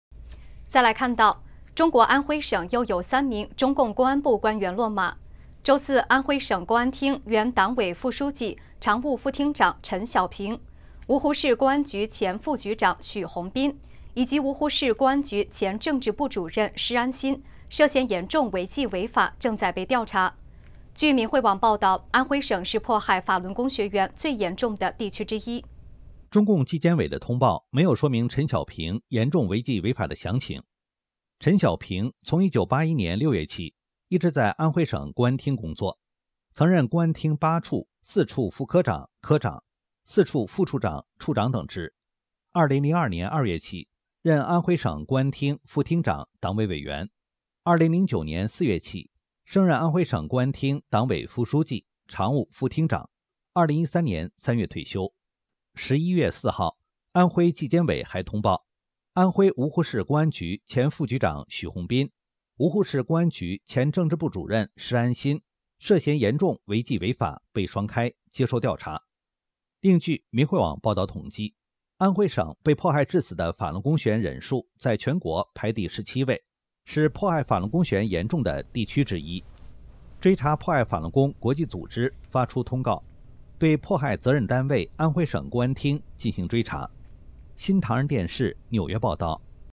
安徽省公安厅副厅长陈小平被查 曾迫害法轮功【停止迫害法轮功 】
【新唐人北京时间2021年11月04日讯】中国安徽省，又有三名中共公安部官员落马。周四，安徽省公安厅原党委副书记、常务副厅长陈小平，芜湖市公安局前副局长许红斌，以及芜湖市公安局前政治部主任施安新，涉嫌严重违纪违法，正在被调查。据《明慧网》报导，安徽省是迫害法轮功学员严重的地区之一。

中共纪监委的通报没有说明陈小平严重违纪违法的详情。

陈小平从1981年6月起，一直在安徽省公安厅工作，曾任公安厅八处、四处副科长、科长、四处副处长、处长等职；2002年2月起，任安徽省公安厅副厅长、党委委员；2009年4月起，升任安徽省公安厅党委副书记、常务副厅长。2013年3月退休。

11月4号，安徽纪监委还通报，安徽芜湖市公安局前副局长许红斌、芜湖市公安局前政治部主任施安新，涉嫌严重违纪违法，被双开，接受调查。

另据明慧网报导统计，安徽省被迫害致死的法轮功学员人数在全国排第17位，是迫害法轮功学员严重的地区之一。

「追查迫害法轮功国际组织」发出通告，对迫害责任单位安徽省公安厅进行追查。

新唐人电视纽约报导